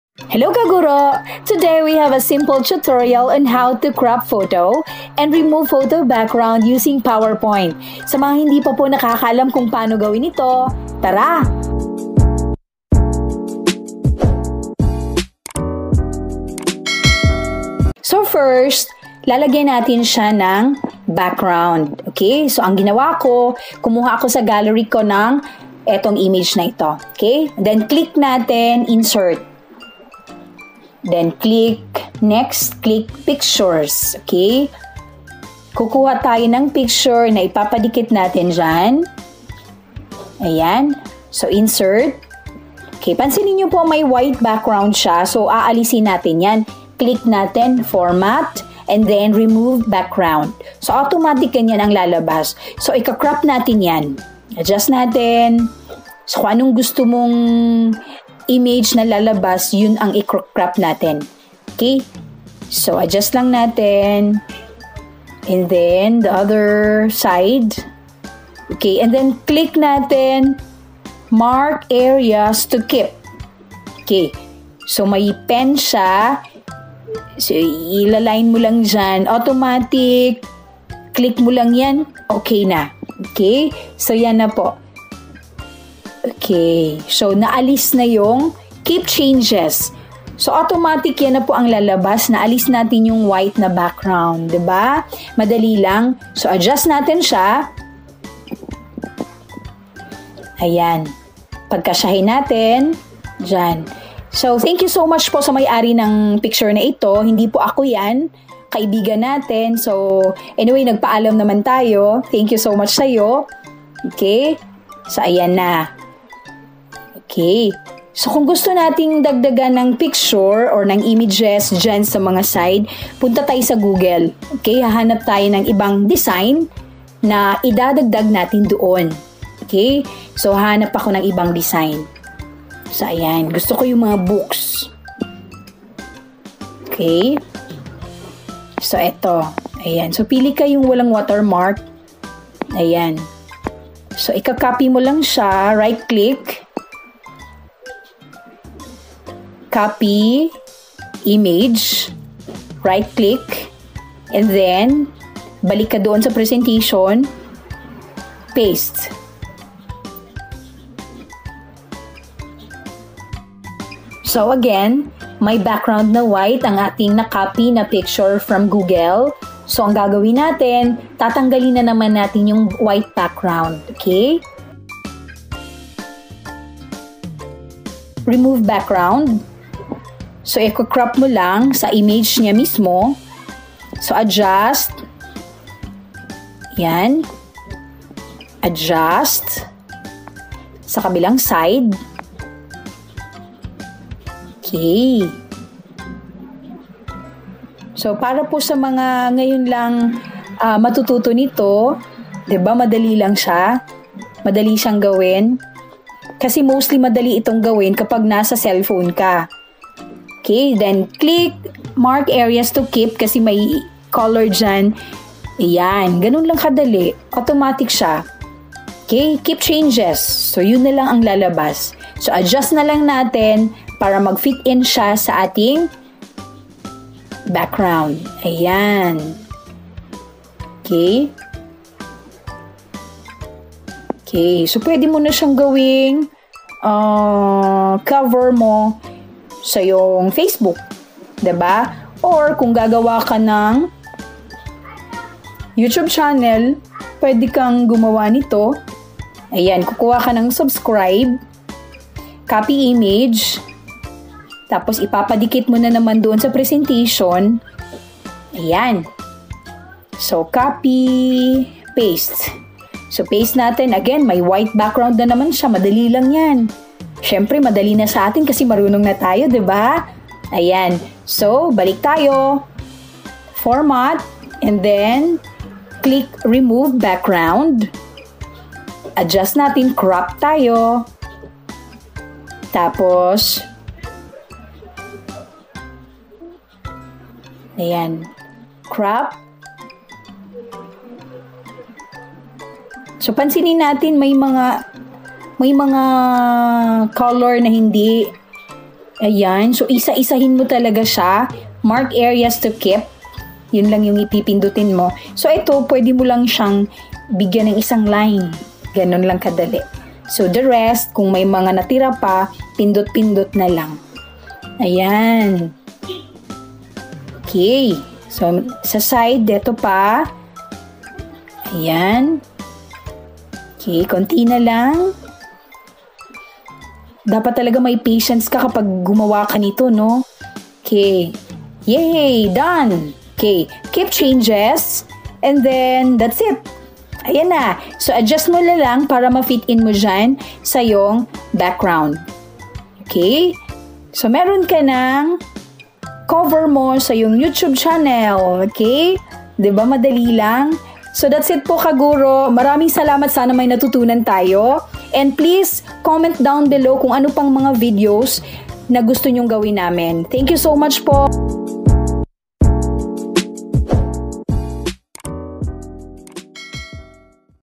Removing Photo Background using PowerPoint- Tagalog Version
Hello kaguro! Here's our simple step-by-step tutorial on how to remove photo background in using PowerPoint.